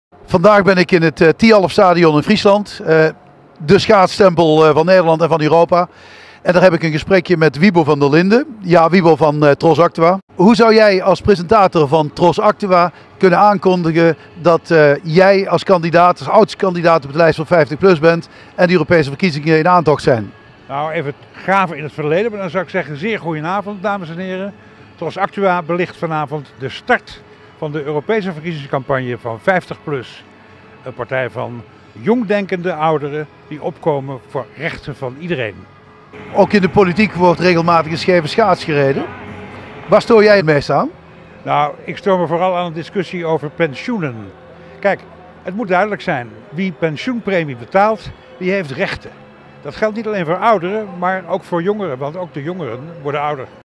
Wibo van de Linde over belang van 50PLUS
Toine Manders bezoekt Wibo van de Linde van Tros Aktua